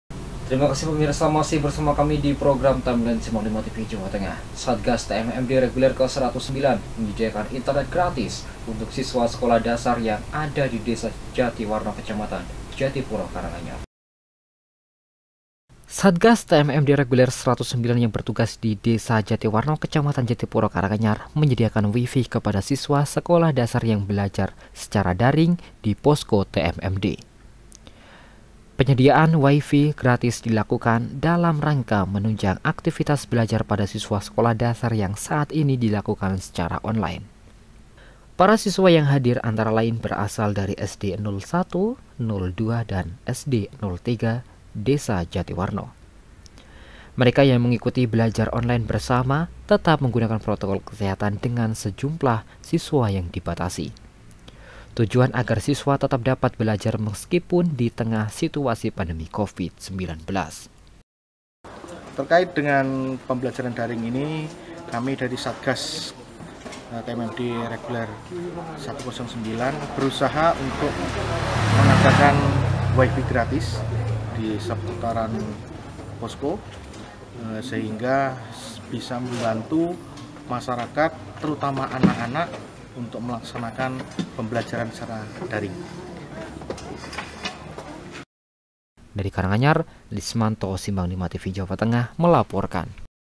Satgas TMMD Sediakan Wifi Gratis untuk Siswa SD Jatiwarno Karanganyar
Satgas Tmmd Reguler Ke-109 Menyediakan Internet Gratis Untuk Siswa Sekolah Dasar Yang Ada Di Desa Jatiwarno Kecamatan Jatipuro Karanganyar.